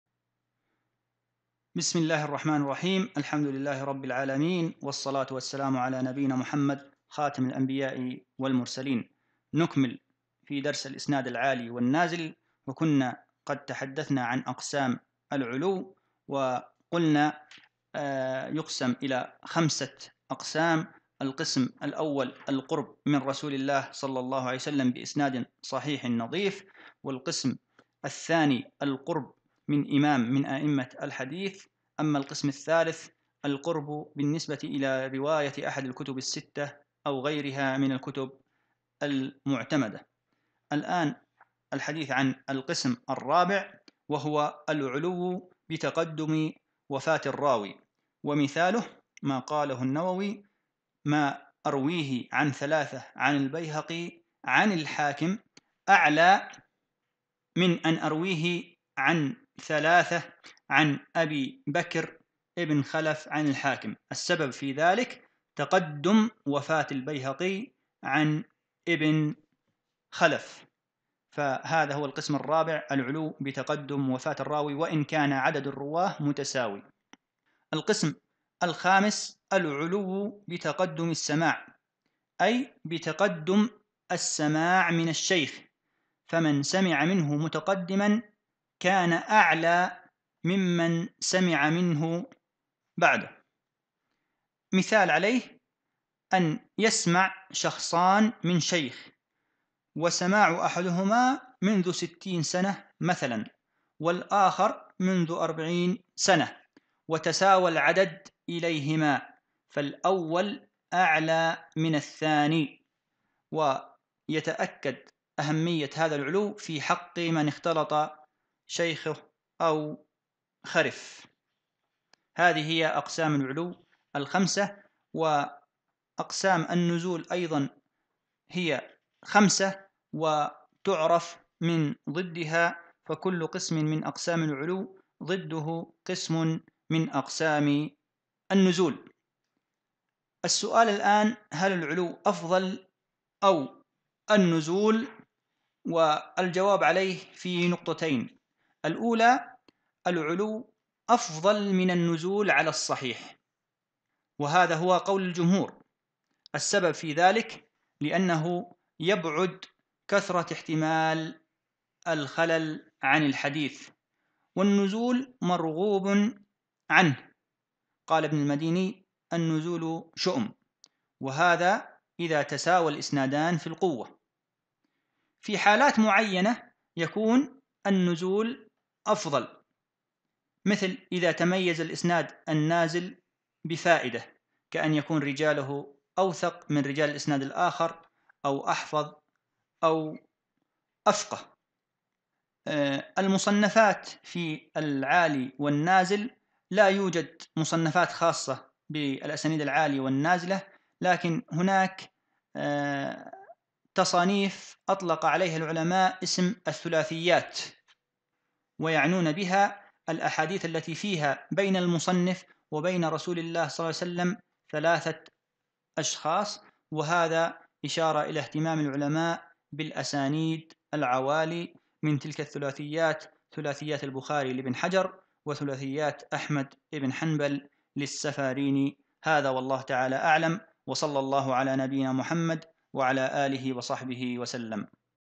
مصطلح الحديث - 3 | الدرس 7-2 | الإسناد العالي والنازل
الدرس 7-2 : الإسناد العالي والنازل
تتمة أقسام العلو، أقسام النزول، هل العلو أفضل أم النزول؟، أشهر المصنفات فيه.